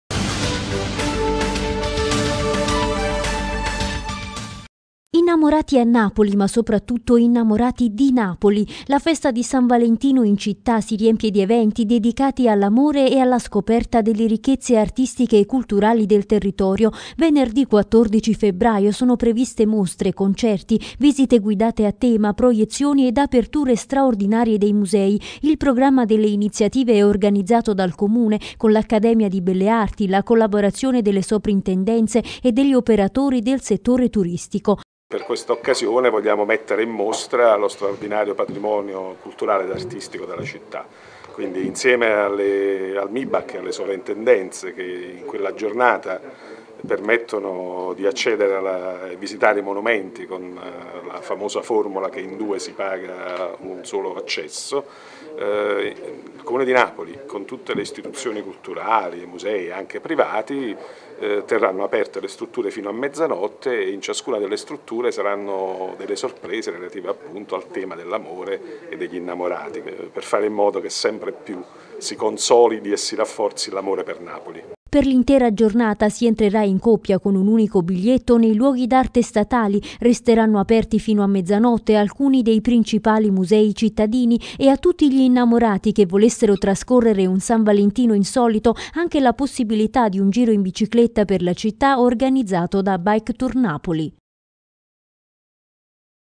Napoli - La Notte degli Innamorati per San Valentino -2- (24.01.14)
- Napoli. Innamorati a Napoli e innamorati di Napoli, tutti insieme per San Valentino, oltre l'orizzonte consueto delle occasioni che propone, ormai per tradizione, la Festa degli innamorati, per catturare l'attenzione di cittadini e turisti con le meraviglie di Napoli, in una notte di manifestazioni culturali diffuse nei nostri muse. La "Notte degli innamorati di Napoli" è un evento organizzato dall'Assessorato alla Cultura e al Turismo, insieme con l'Accademia delle Belle Arti ed in collaborazione con le Soprintendenze, i musei, istituzioni culturali ed operatori del turismo. (24.01.14)